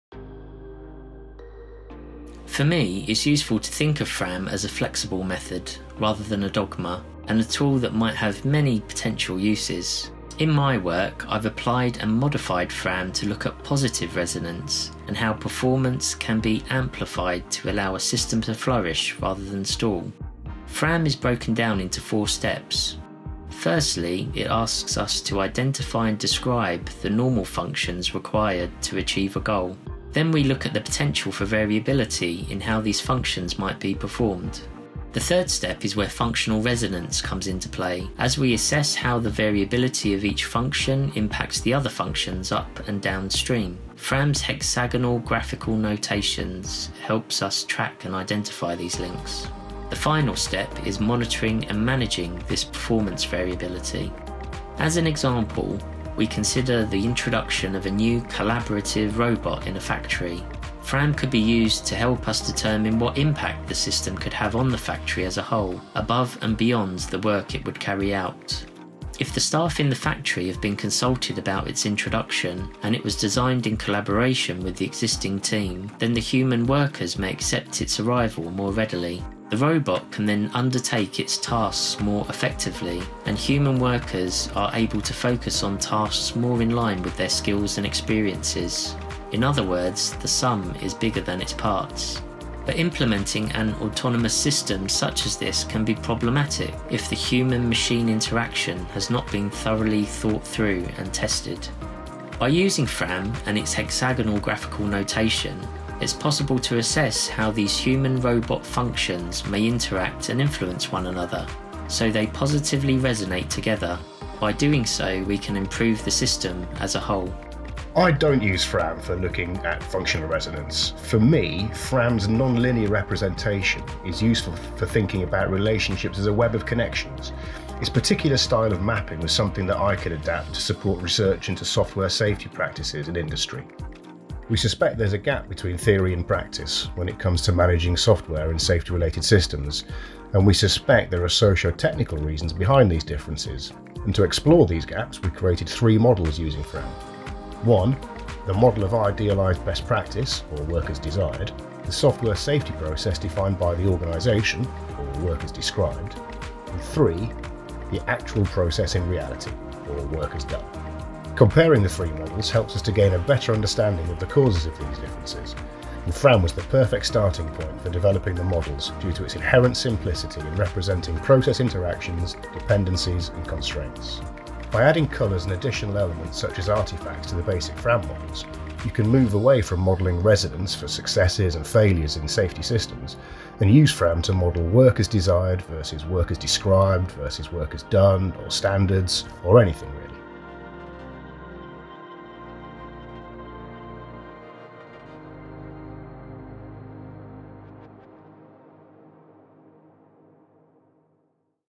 The flexibility of the Functional Resonance Analysis Method (FRAM)
FRAM was developed by Erik Hollnagel. This video, created by the Assuring Autonomy International Programme and Dr Dominic Furniss, looks at how flexible the method is and how it can be used in different ways.